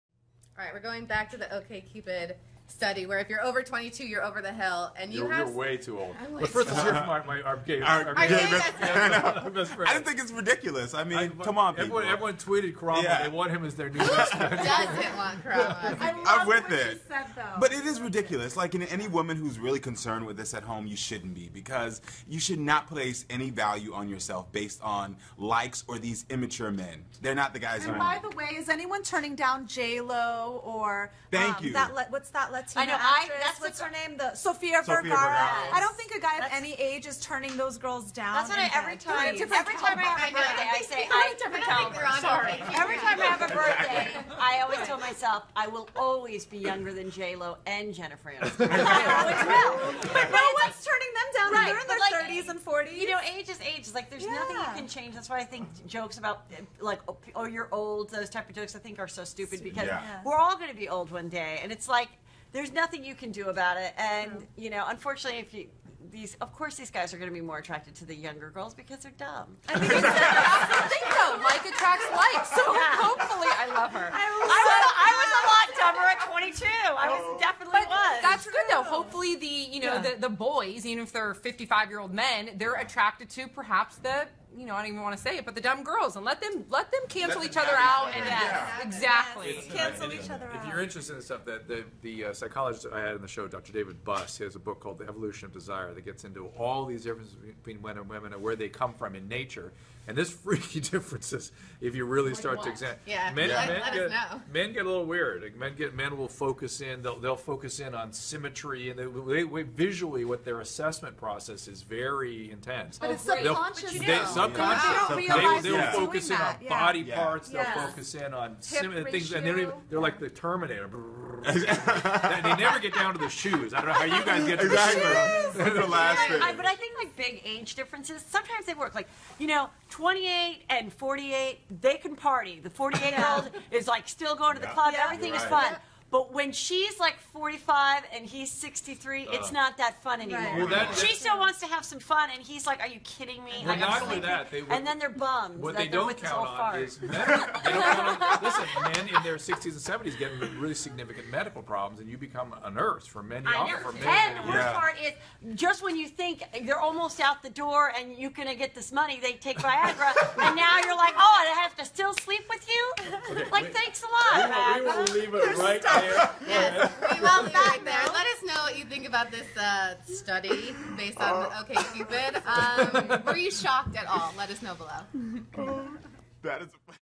After-show: Are you shocked by OKCupid data?
OKCupid president and co-founder, Christian Rudder, is sharing some shocking statistics in his new book, "Dataclysm: Who We Are When We Think No One's Looking."